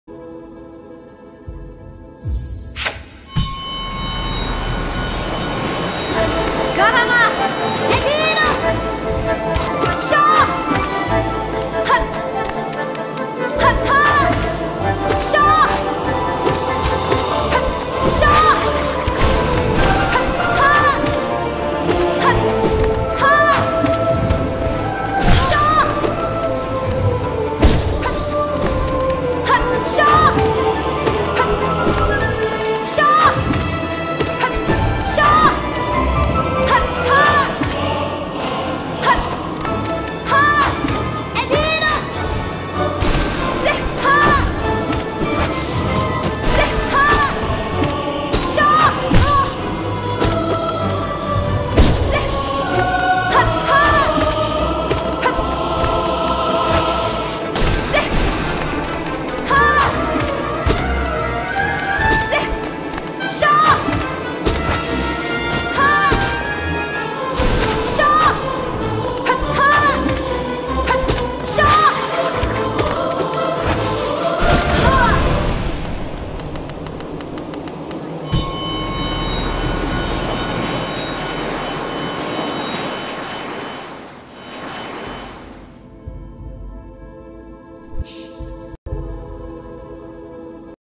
Flawless Hornet Sentinel
I am now officially 1/4 of the way through getting flawless on every boss.